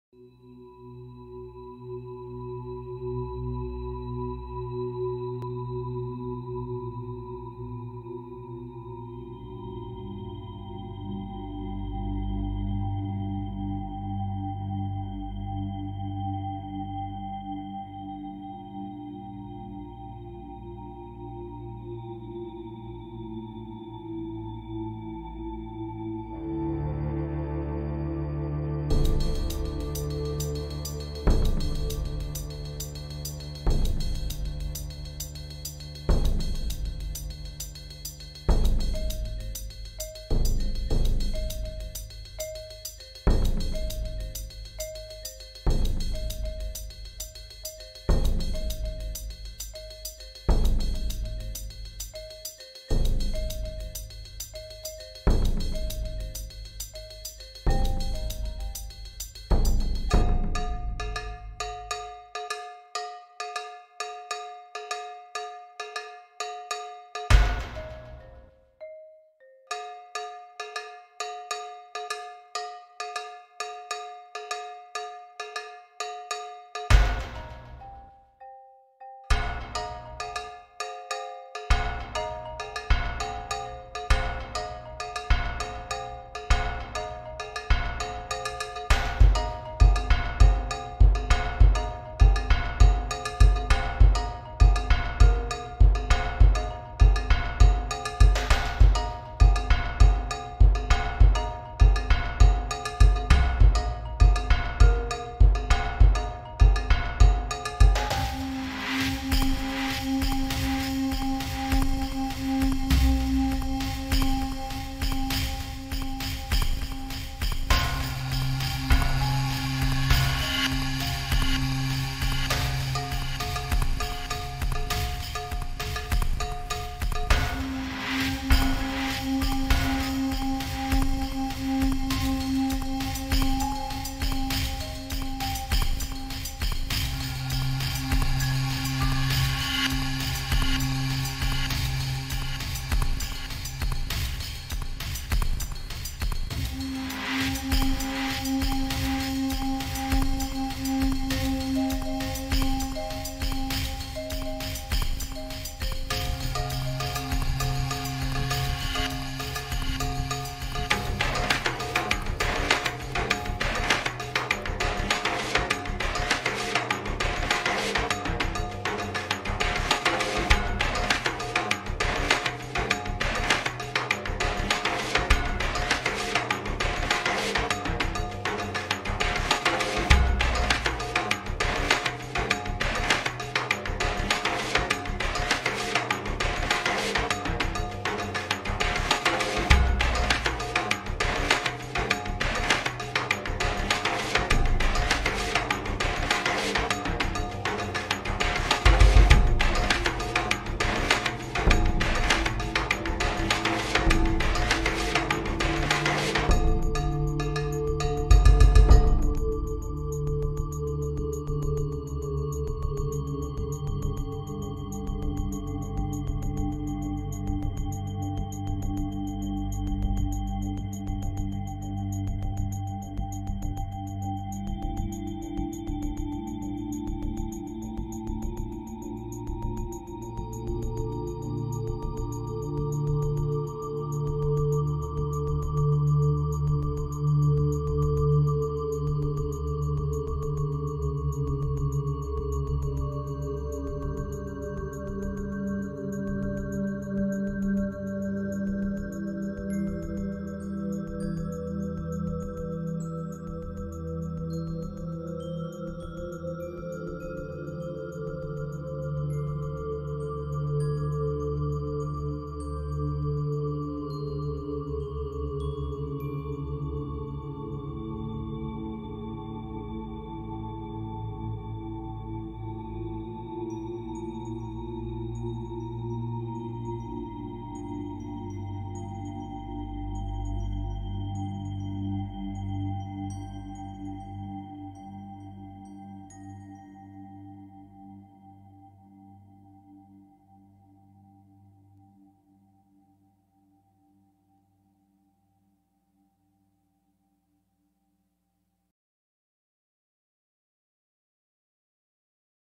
Big Cats of the Jungle
Scenes of big cats in the jungle and around the world.